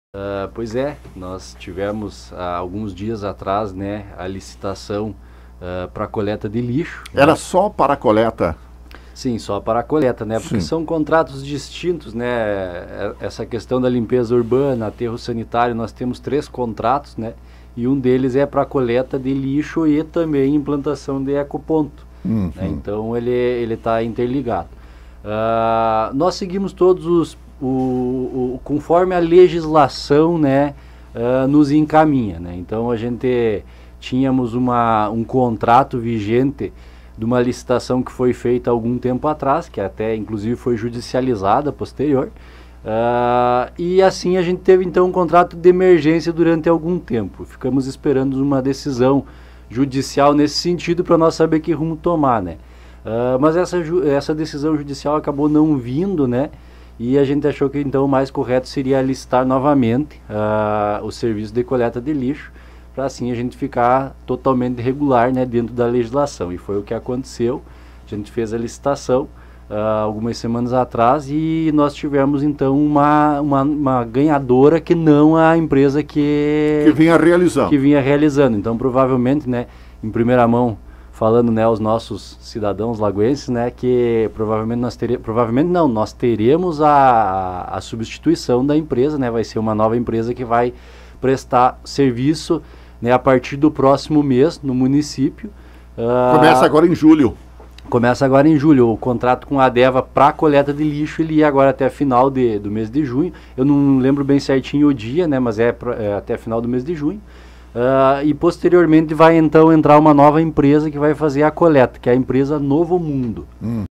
Nova empresa realizará coleta de lixo em Lagoa Vermelha
A Administração municipal de Lagoa Vermelha promoveu licitação para o serviço de coleta de lixo do município. Esse serviço vem sendo prestado pela Adeva. Uma nova empresa foi a vencedora da licitação. É a Novo Mundo. É somente para coleta de lixo. O serviço de limpeza continua com a Adeva. Foi o que informou o vice-prefeito e Secretário Geral da Administração, Eder Piardi: